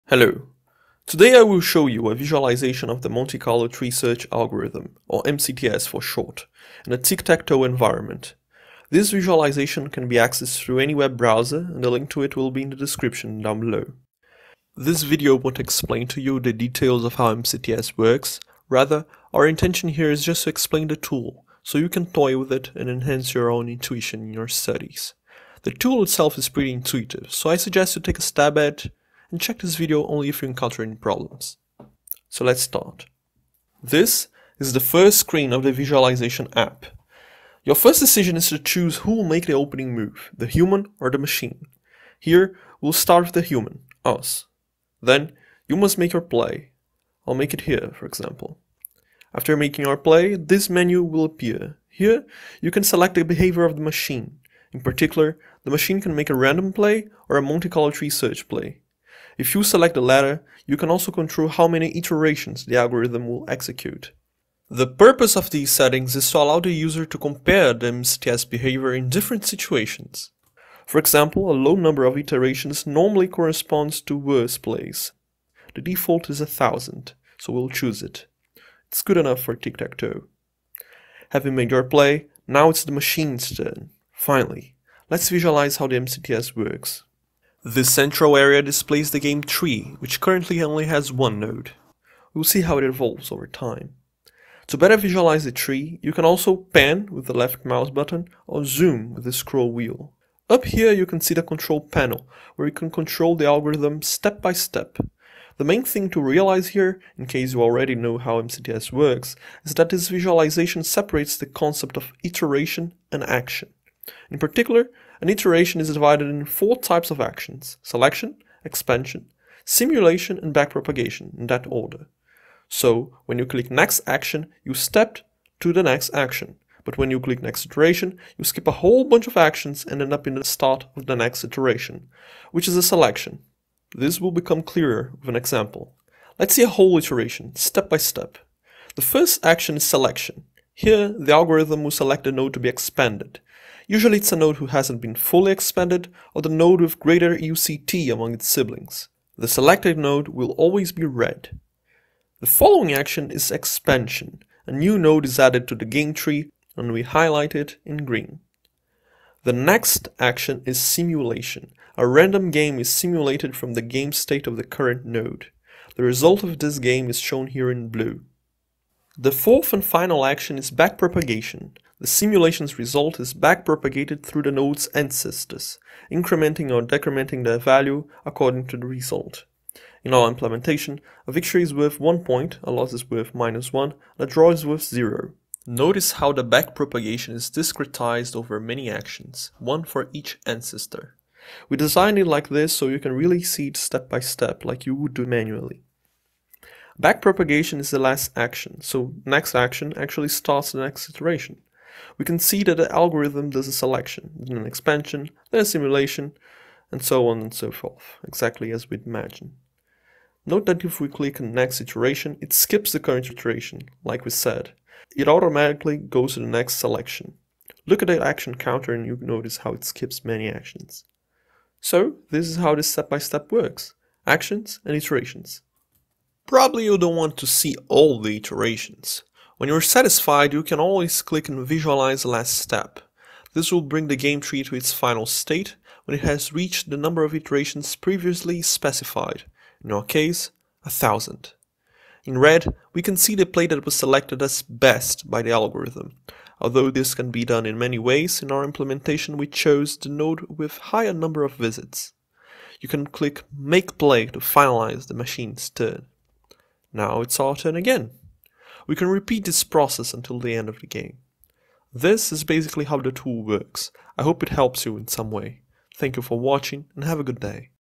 Monte Carlo Tree Search - Tic-Tac-Toe Visualization
A visualization of the Monte Carlo Tree Search algorithm, applied to Tic-Tac-Toe.